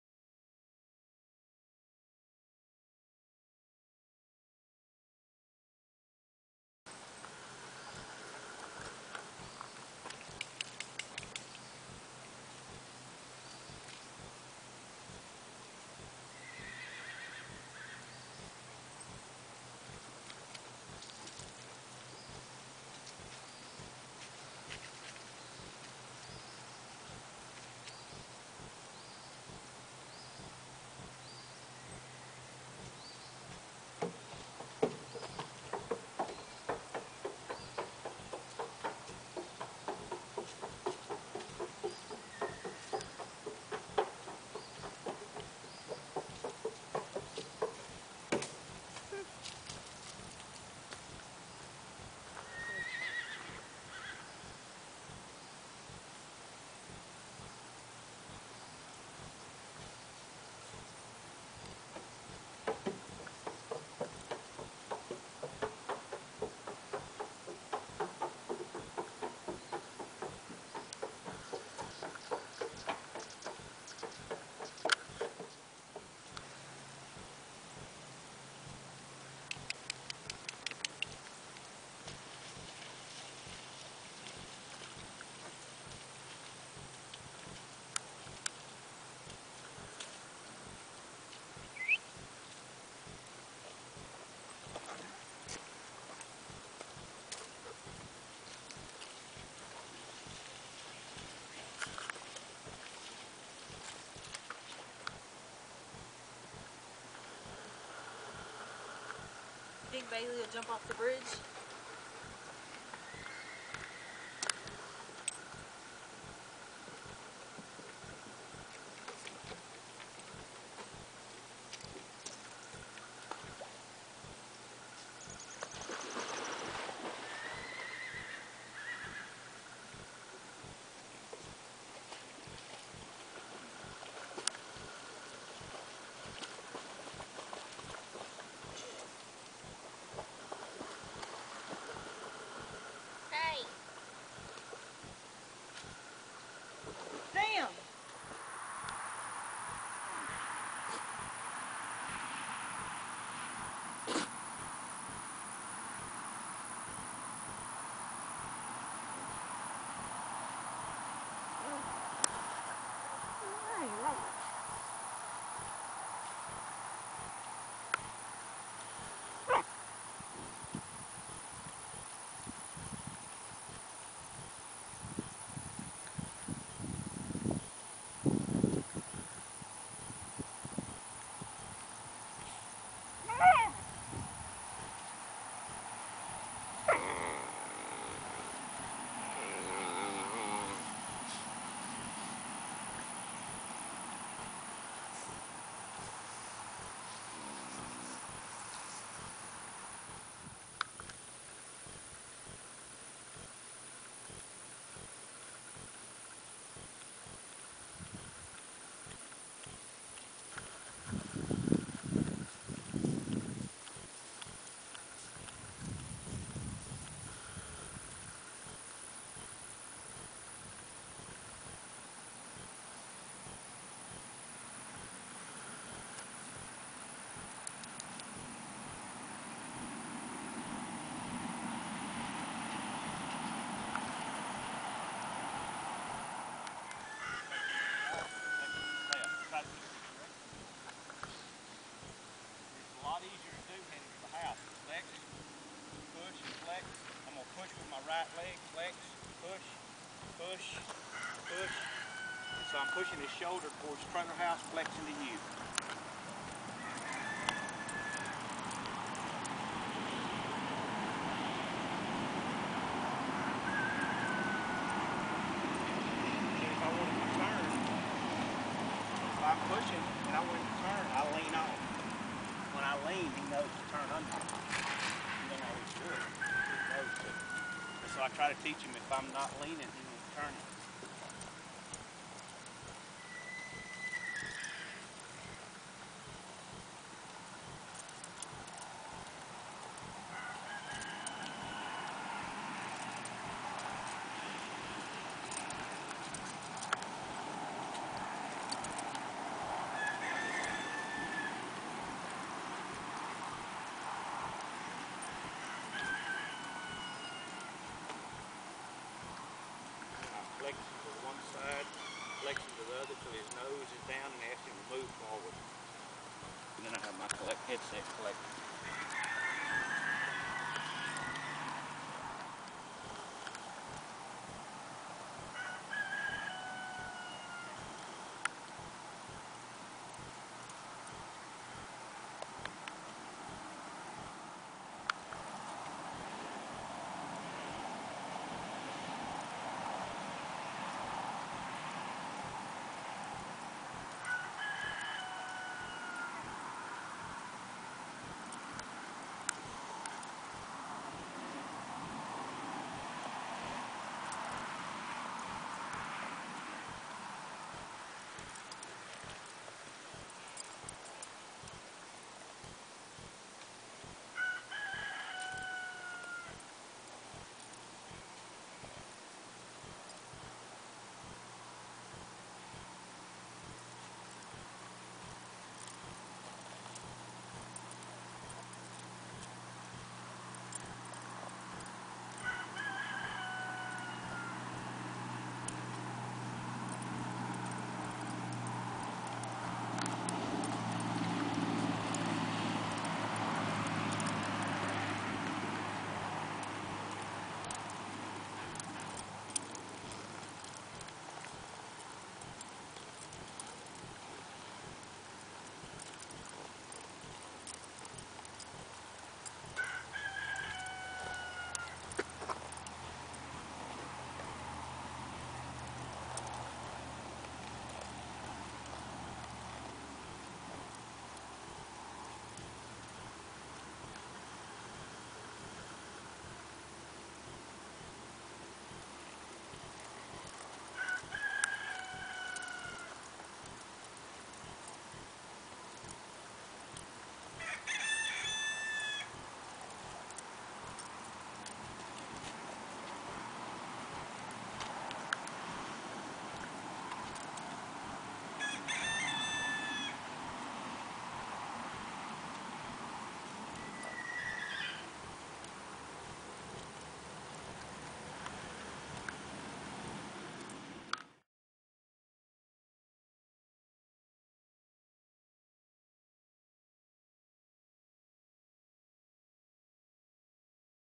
Unusual Strikingly Flashy Classic Champagne Tobiano Tennessee Walking Horse Gelding for sale
Nice, nice, athletic, smooth-gaited Tennessee Walking Horse, lots of natural horsemanship training, started the right way.  BAREFOOT, by the way!